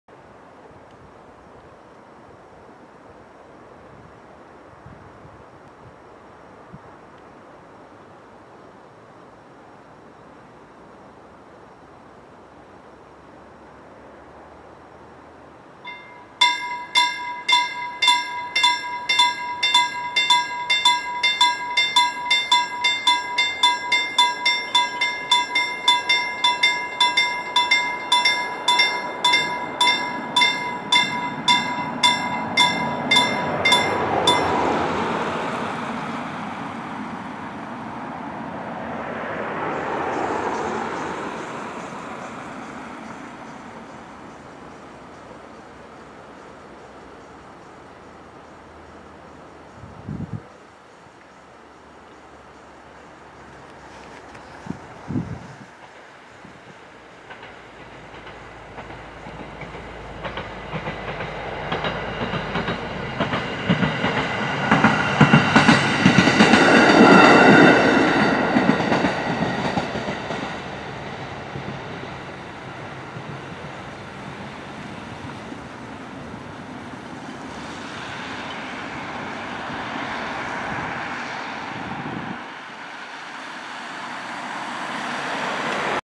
Ttr1 51 Kaanaantiellä
Katso parhaita rataosavideoita:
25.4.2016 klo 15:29

Radantarkastusvaunu Ttr1 51 "Emma" junassa 73051 Naantalista Turku asemalle ylittää Kaanaantien puolipuomilaitoksen Raisiossa.